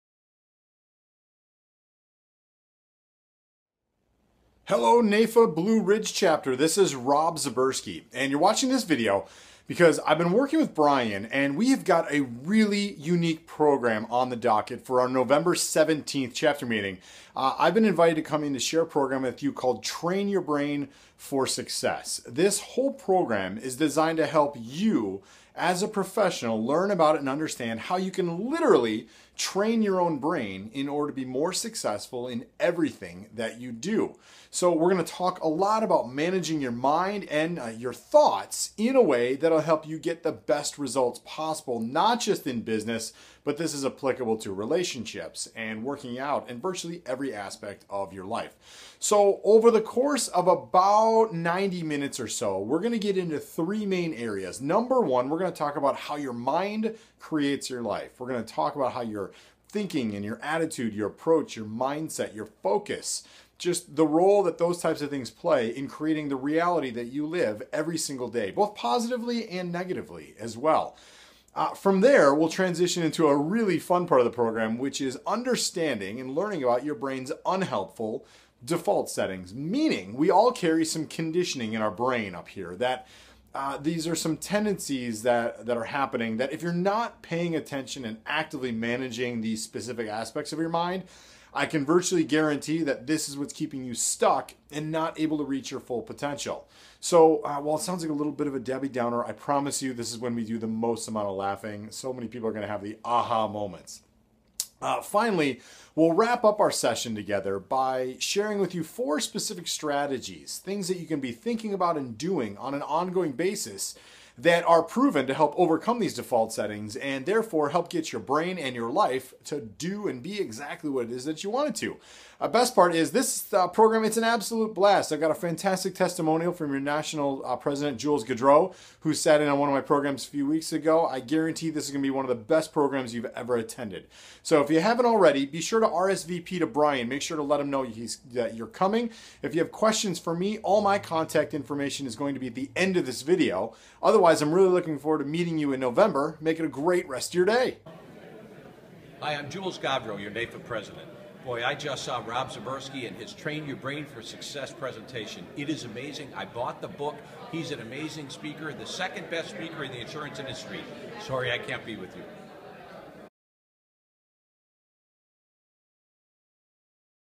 NAIFA Blue Ridge TYBFS Intro
Here's a quick video to introduce you to the Train Your Brain For Success program we'll be sharing on November 17.  See you soon!